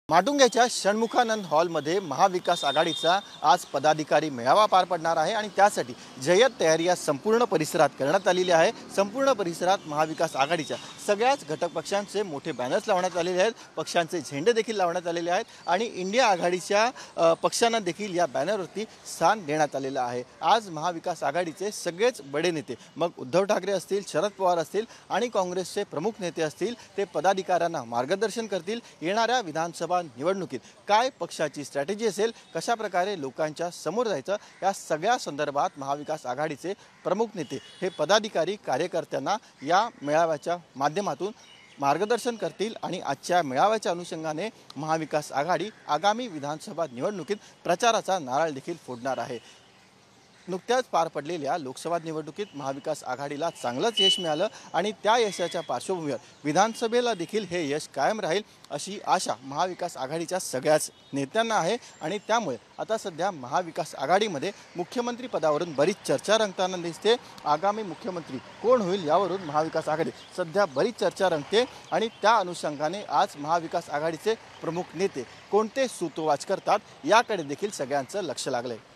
मुंबईत मविआचा मेळावा ,बोलके बॅनर्स आणि INDIA आघाडी।MVA News।
आज मुंबईत षण्मुखानंद हॉलमध्ये महाविकास आघाडीचा मेळावा पार पडणार आहे. यावेळी इथे लावलेल्या बॅनर्सची चांगलीच चर्चा पाहायला मिळाली. आता मविआच्या मेळाव्यात नेते नवीन कोणते सूतोवाच करणार याकडे सगळ्यांचं लक्ष लागलं आहे.